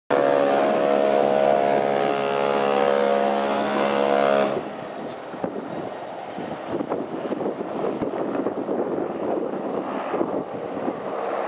Queen Elizabeth Sounds foghorn entering Southampton Water for the first time 08/10/2010
Queen Elizabeth Sounds foghorn entering Southampton Water for the first time